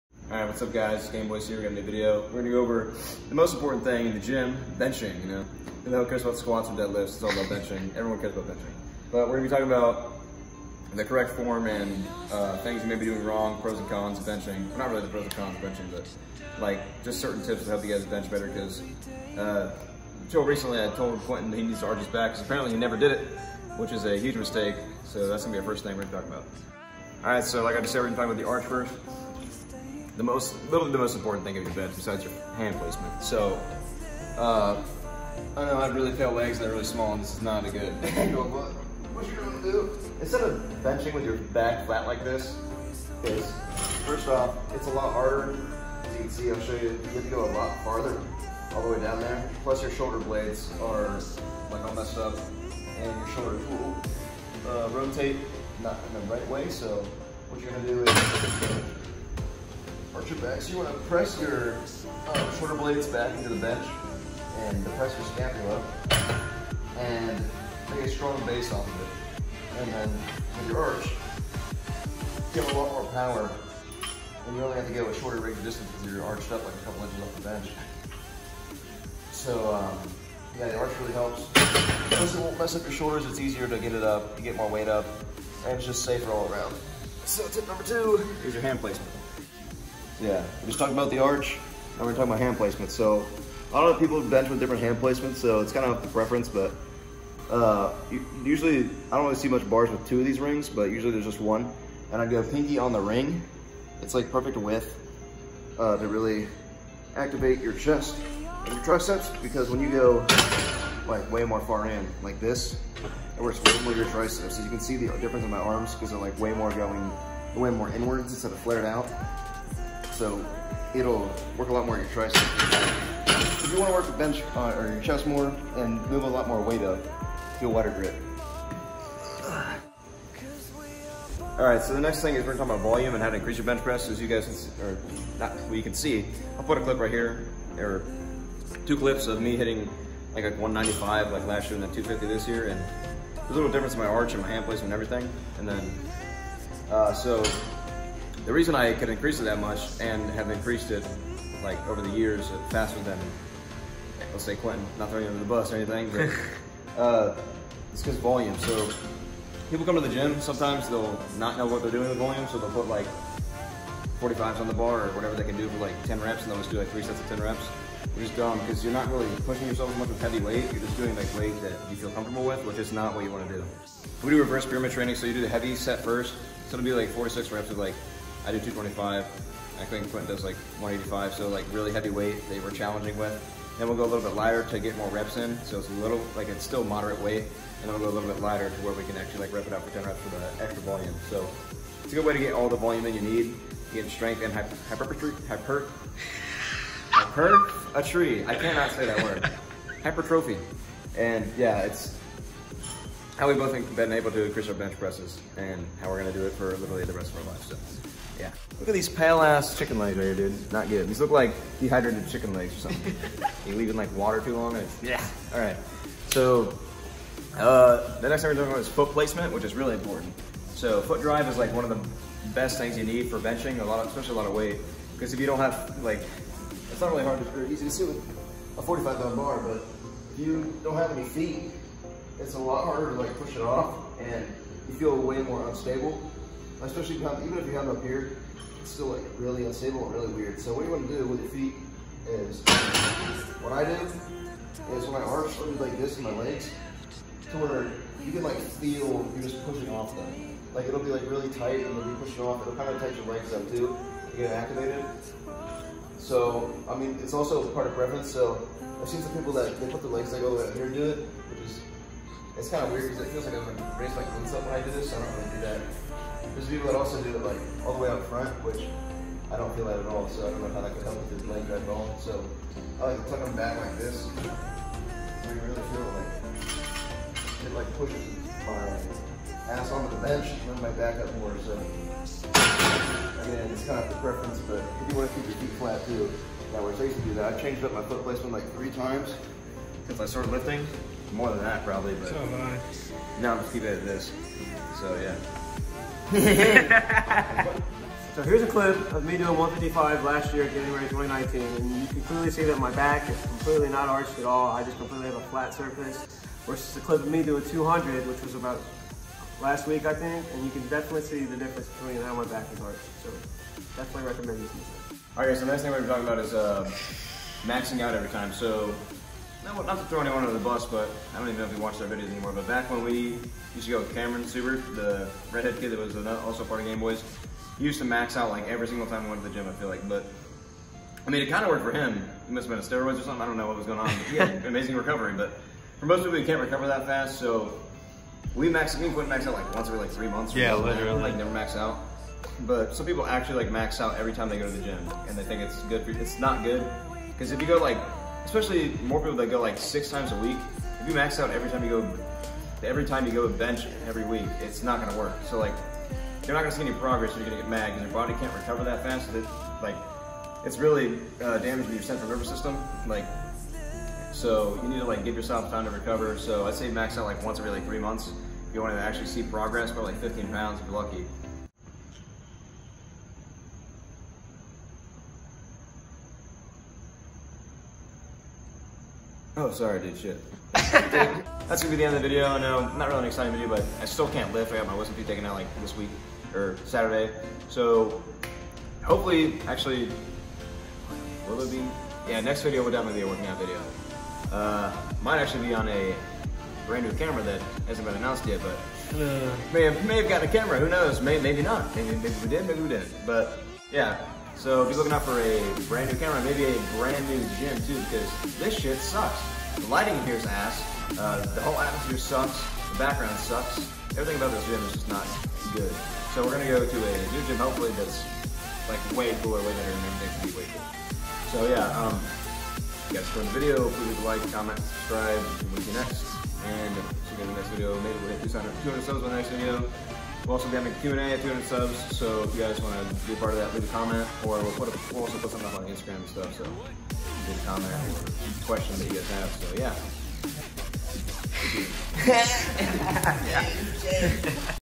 How To Bench Press
In today’s video Jacob and Q go over a few tips for a better bench, and how to properly bench press.

If you Enjoyed the Video leave a Like it helps out a lot!

Jacob- @thee_only__asian
Quentihn- @_quentihn
Dom- @dom.herrick

We here at Gain Boys take pride in helping the common person achieve their goals in the gym and with their lifestyle.